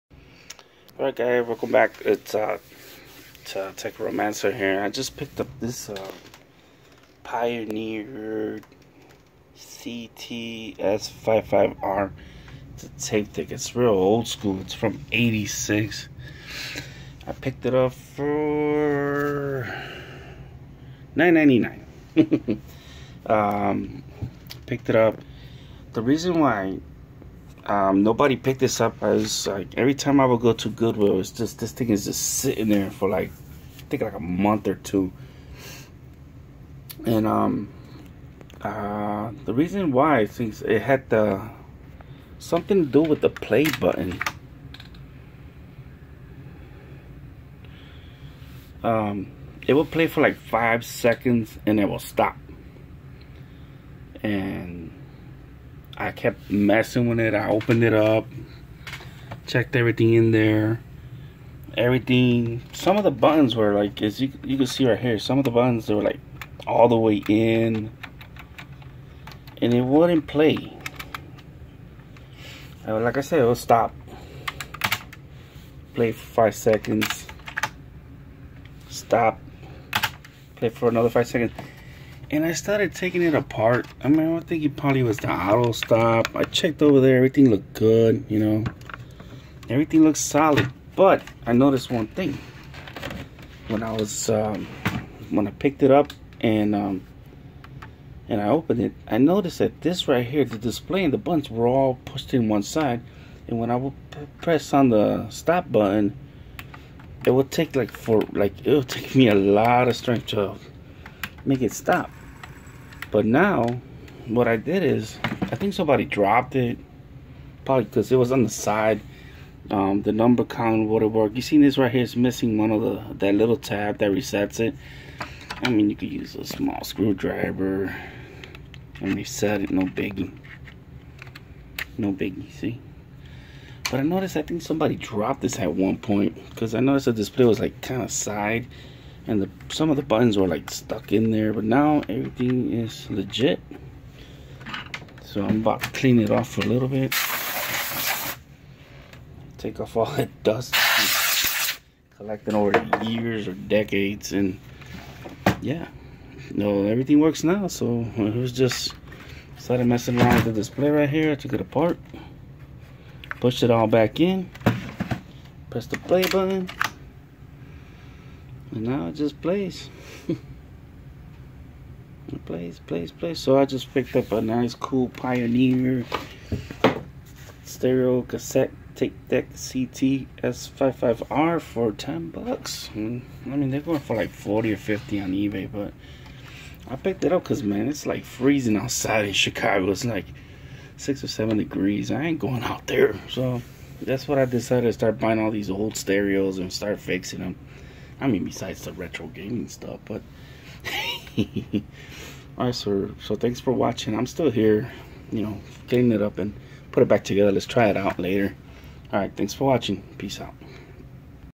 Pioneer ct-s55r
It's cold out there time to create a new hobbie fix up oldschool stereo equipment